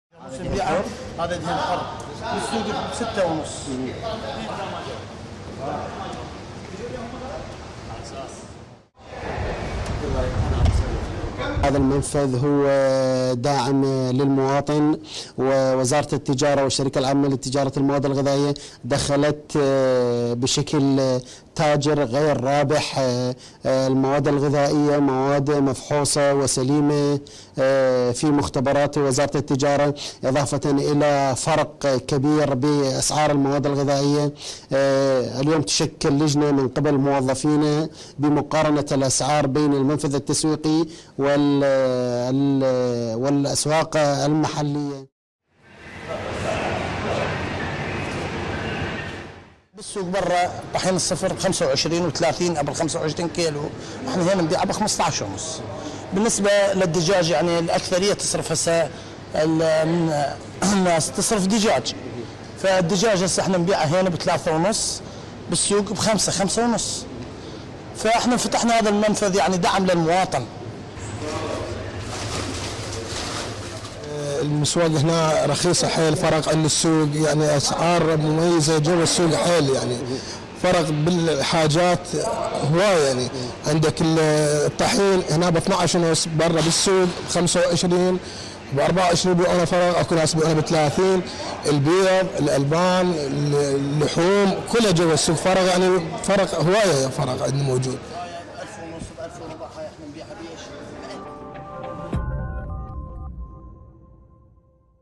“أورزدي” مصغر في تكريت.. منفذ التجارة المدعوم يبيع الغذائية “جوا السوگ”
التجربة ستنتقل إلى بقية المدن
فيديو: “أورزدي” مصغر في تكريت.. منفذ التجارة المدعوم يبيع الغذائية “جوا السوگ”

افتتحت وزارة التجارة في تكريت أول منفذ لبيع السلع الغذائية المدعومة، ضمن خطة حكومية لافتتاح فروع مماثلة في بقية المدن.

حسين محمود – مدير فرع تجارة صلاح الدين، لشبكة 964:
وزارة التجارة دخلت سوق المواد الغذائية في صلاح الدين كتاجر غير رابح.

افتتحت وزارة التجارة، أول منفذ تسويقي أمام المواطنين في صلاح الدين، لبيع مواد غذائية بأسعار مدعومة.

الفارق كبير بأسعار المواد الغذائية بين هذا المنفذ والسوق المحلية.

مجيد عبد – مسؤول المنفذ:
المبادرة مهمة جداً لأصحاب الدخل المحدود، فأسعار أغلب المواد الغذائية أقل بنحو الثلث مقارنة بالأسواق التجارية.

قنينة الزيت مثلا سعرها في الأسواق التجارية 3 آلاف دينار، بينما يبيعها المنفذ بالفي دينار، وهكذا بالنسبة لباقي المواد الأساسية.

براء خالد – متبضع:
الأسعار ممتازة في هذا المنفذ، والفارق كبير مع الأسواق التجارية.

سعر الطحين كمادة أساسية مطلوبة 12 ألف و500 دينار، بينما يباع في الأسواق بـ25 أو 24 ألف دينار.